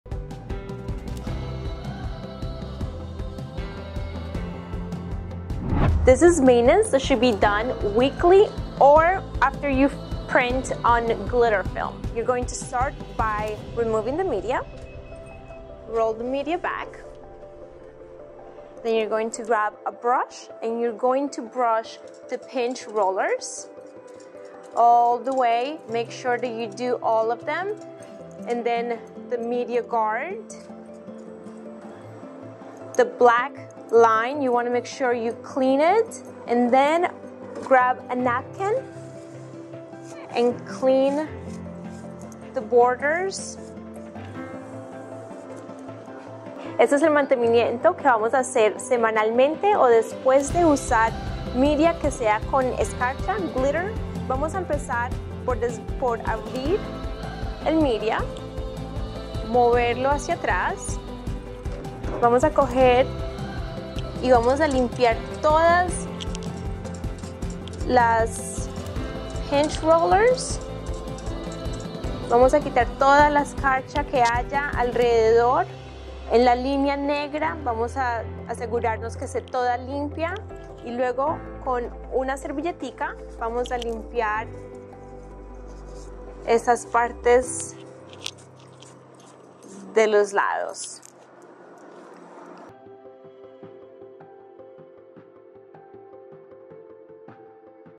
STSinks DTF 628/724 | Glitter film maintenance & Heat platen maintenance (English + Spanish)
In this video, we cover essential maintenance tips for using glitter film and keeping the heat platen clean on the STS Inks DTF 628/724 printer. Whether you're ensuring smooth printing or extending the lifespan of your equipment, these steps are crucial. Watch in both English and Spanish for your convenience!

STSINKS - Color is in Our DNA